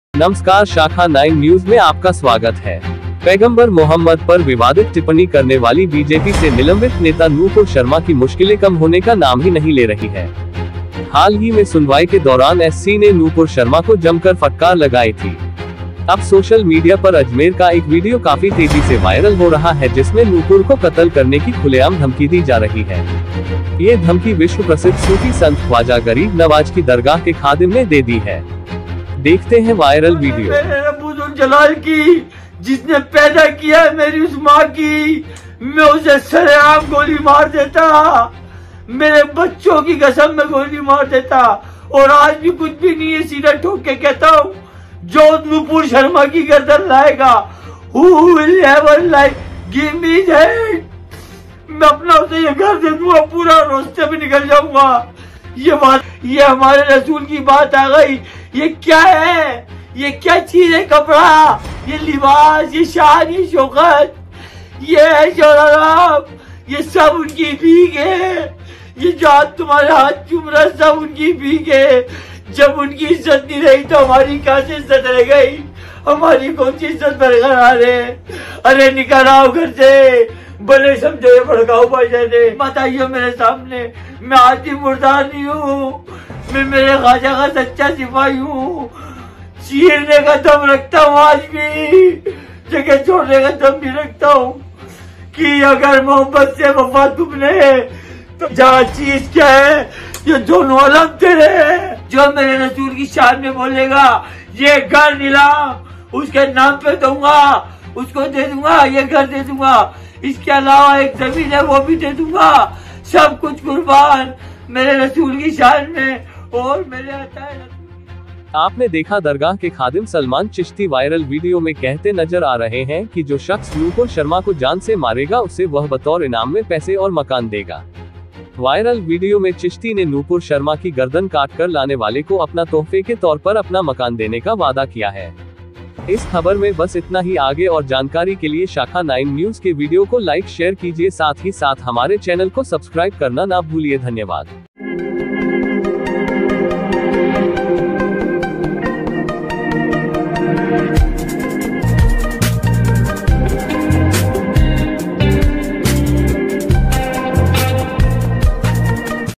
AJMER (अजमेर) दरगाह के खादिम सलमान चिश्ती का भड़काऊ बयान , POLICE तलाश में जुटी / Viral Video
Shakha9 News